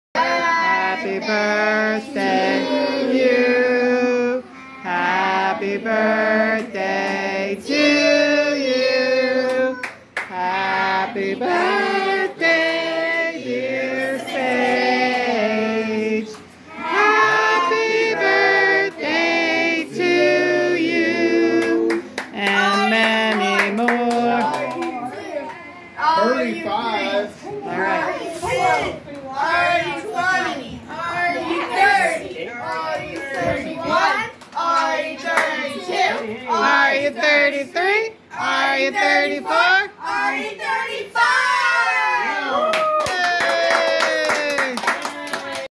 Happy Birthday Saige
We finally got together to celebrate with Saige. Her birthday is in June but we celebrated today